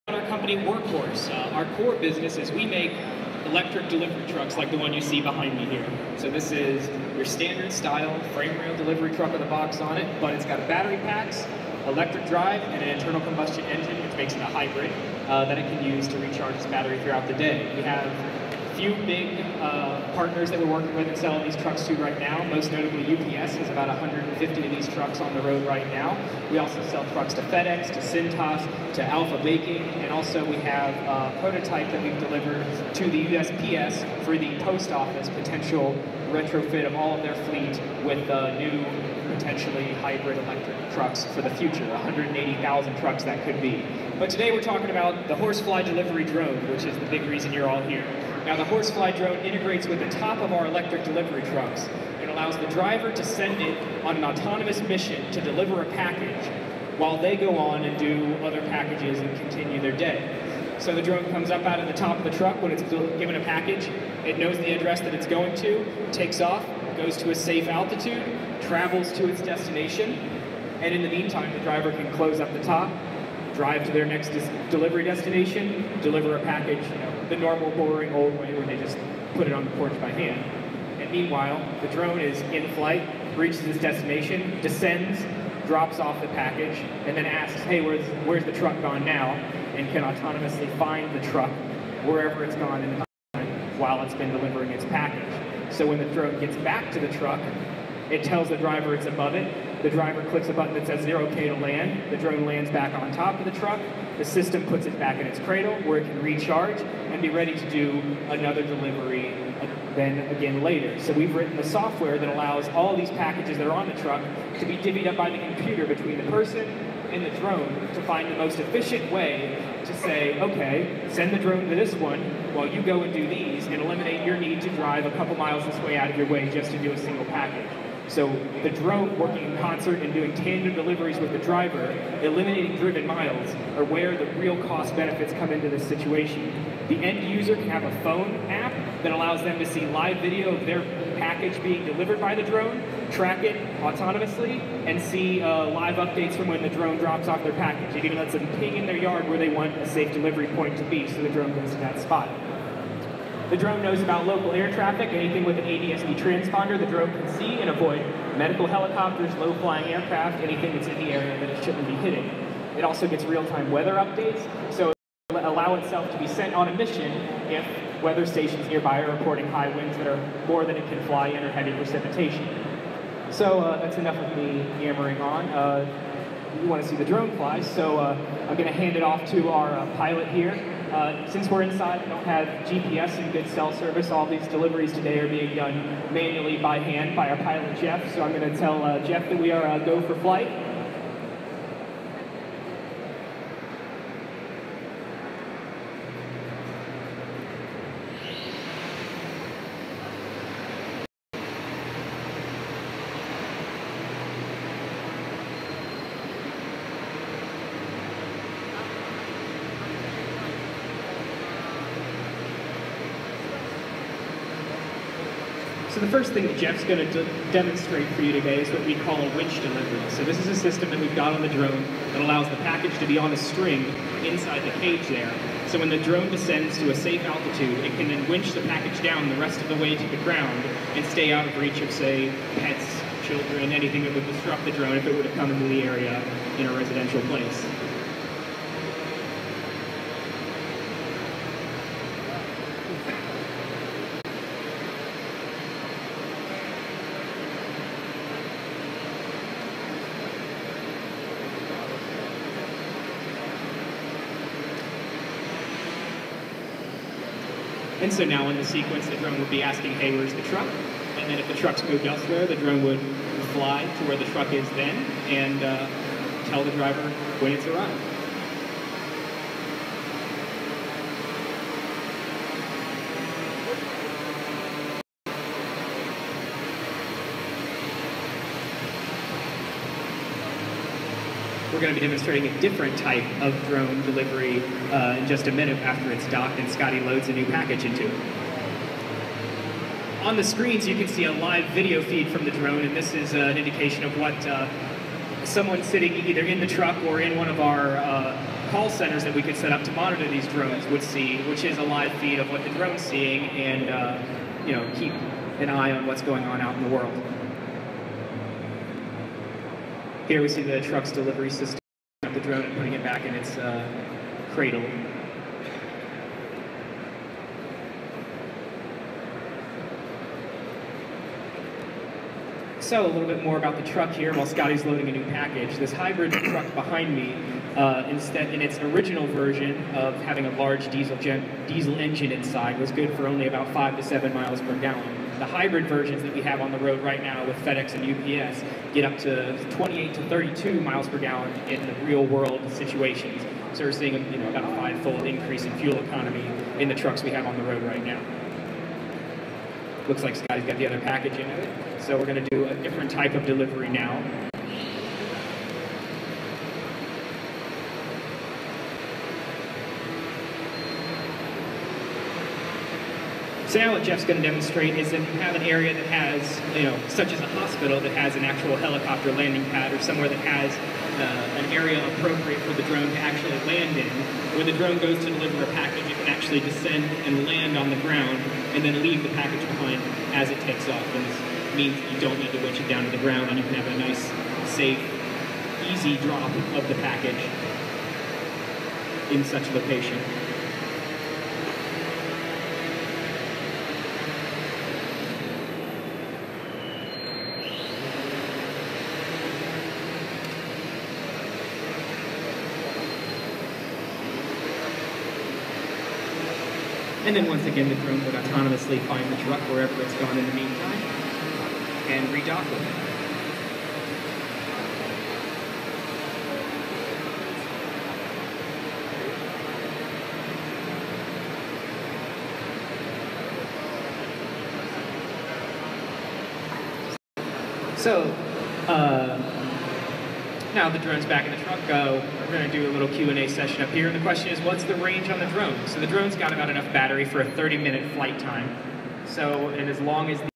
Workhorse Drone
Live Stream from Pack Expo Las Vegas 2017. Super cool package delivery drone.